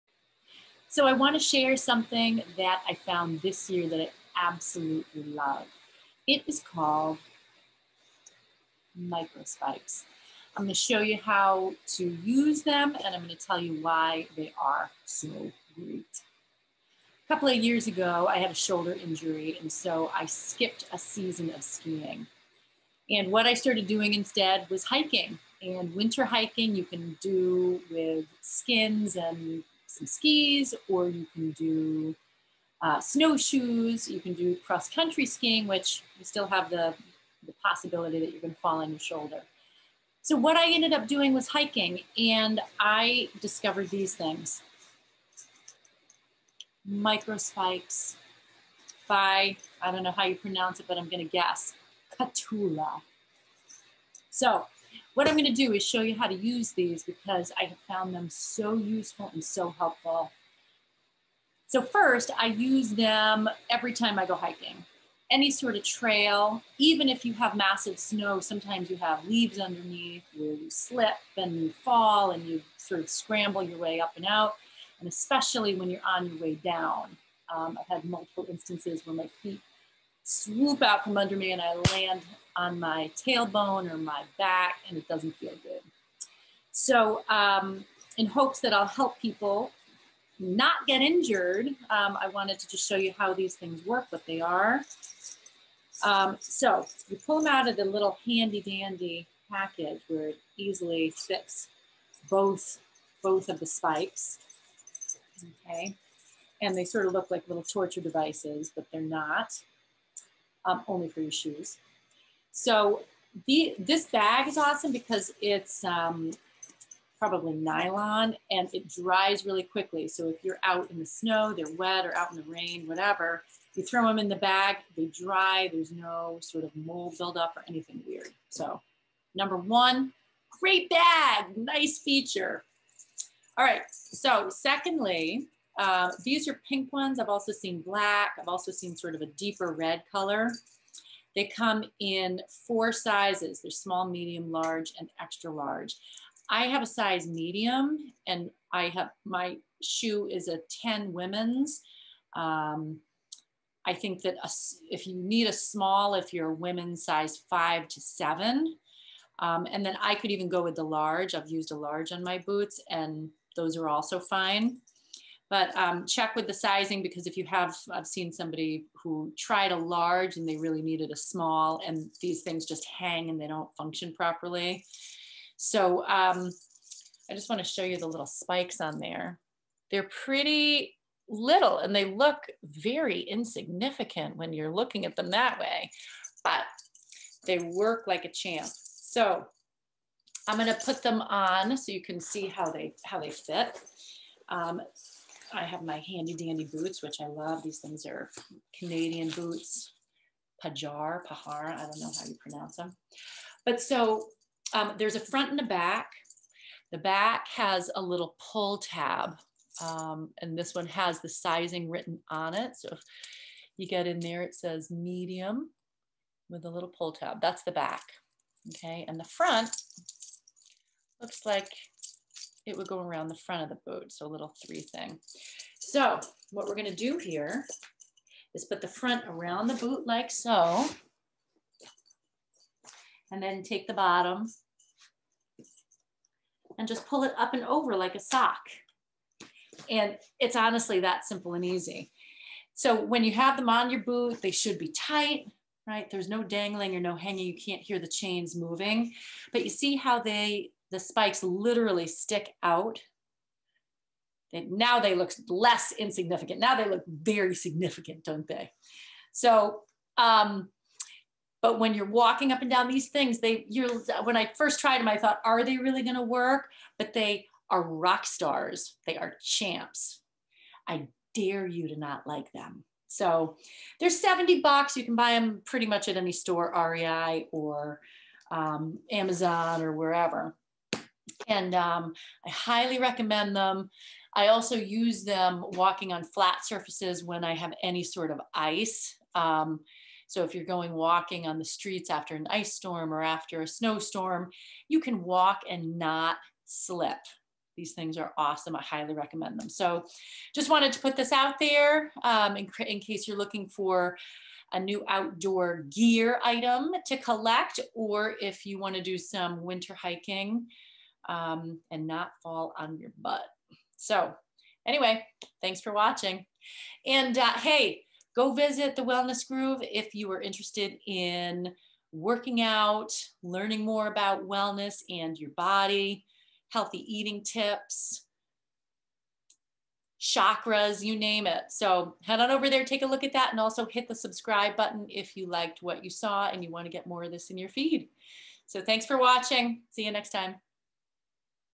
5 Min - Microspikes
Microspikes are my new favorite winter gear item. Learn why I love them so much, see how to put them on, and how to use them! You will need a pair after you watch this video!

for wellness tips, motivation, and on-demand & live training.